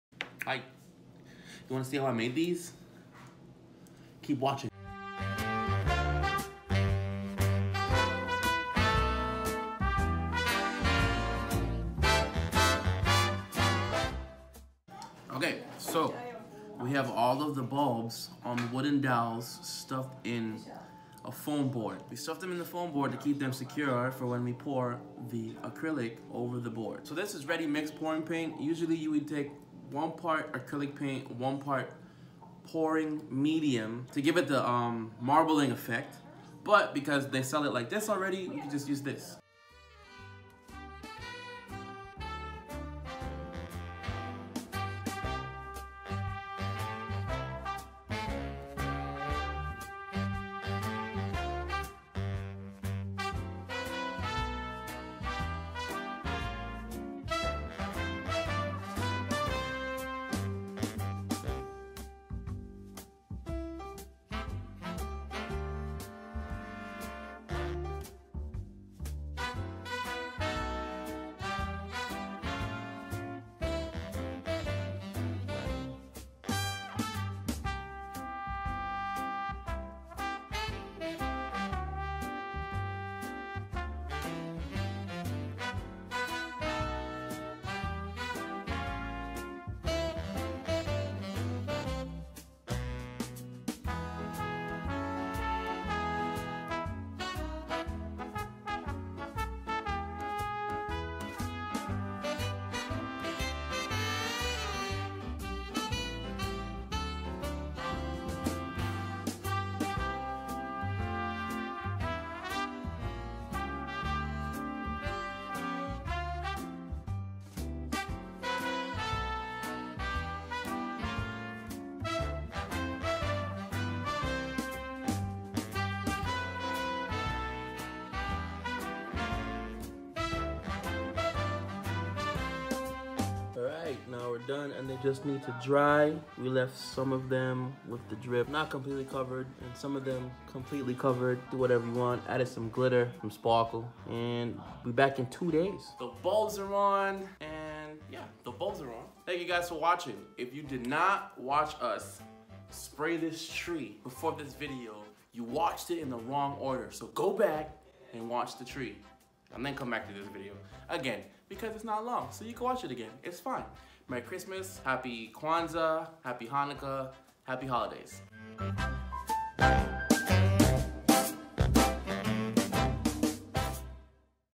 Vlogmas Day 4 : DIY Marble Ornaments
Day 4 Letsgetit!

For this DIY Project you'll need:
Acrylic Paint
Pouring Medium
Clear Ornament Bulbs
Wooden Dowels
Glue gun
Foam Board

Shoutouut to my momma and my aunt for helping us pour.